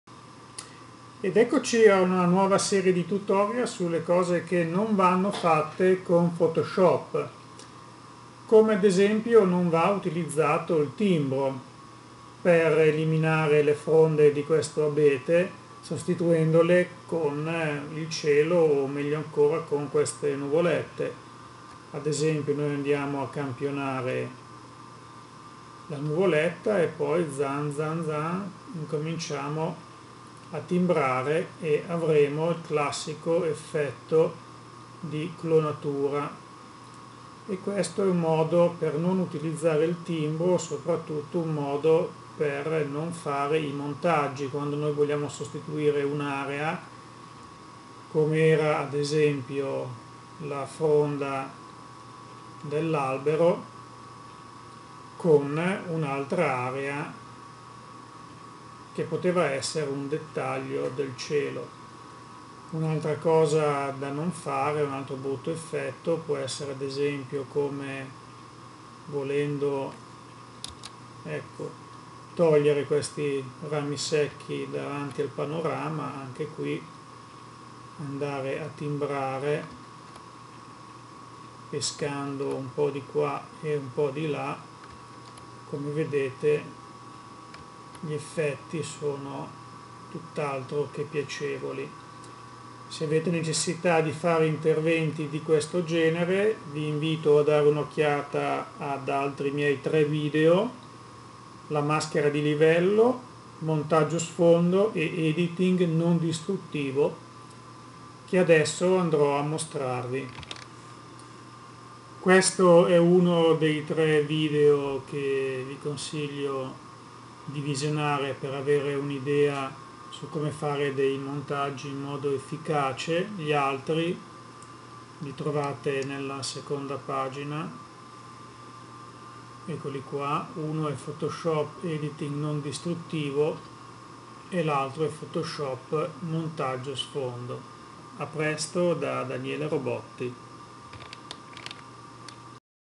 IL TIMBRO: COME USARLO MALE
Come non fare un montaggio usando il Timbro Clone di Photoshop.